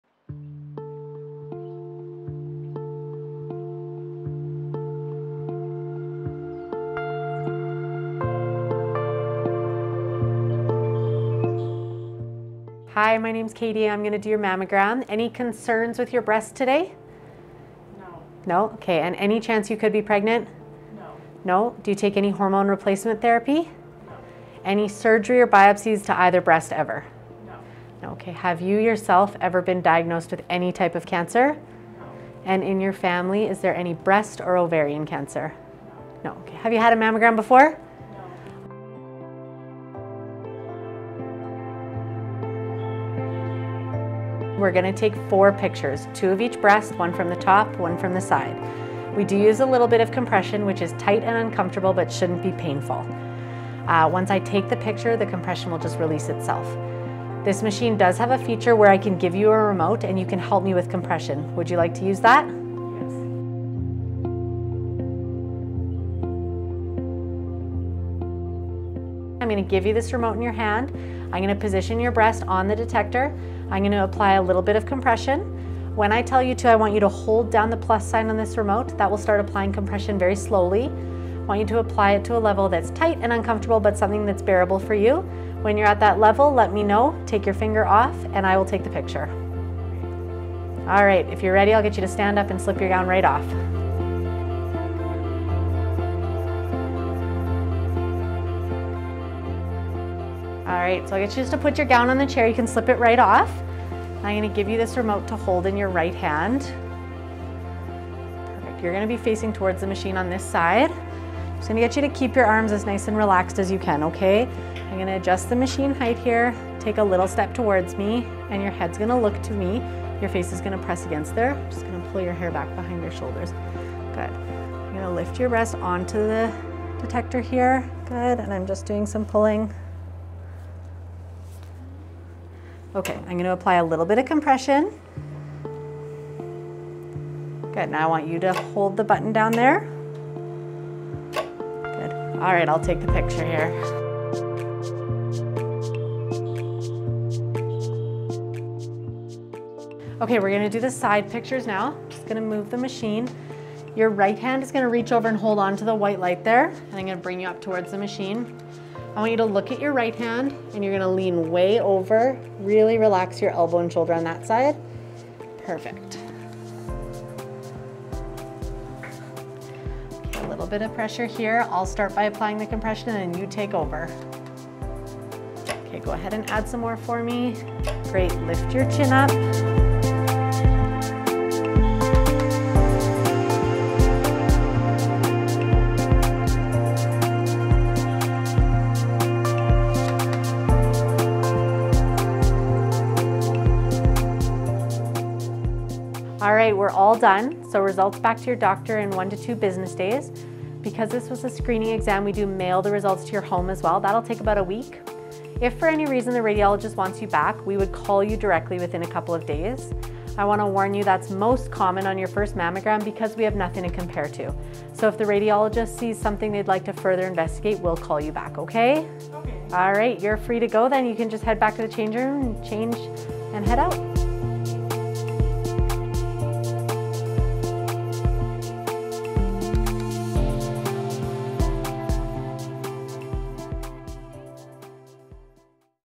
What Happens During a Mammogram?
Regular screening mammograms are the best way to detect breast cancer in its early, more treatable stage. Having regular mammograms makes it easier to compare your images and can reveal changes that are too small for you or your doctor to feel.

Mayfair Diagnostics recommends screening mammography every year from age 40 to 49. Between ages 50-75 regular mammograms are recommended every two years if there are no risks factors that would necessitate a shorter interval, such as a family history of breast cancer or extremely dense breasts. After age 75, screening frequency will depend on a number of factors, including your medical history.

However, many women dread having these exams and delay booking an appointment. We have created a video to show what happens during a mammogram to help reduce anxiety.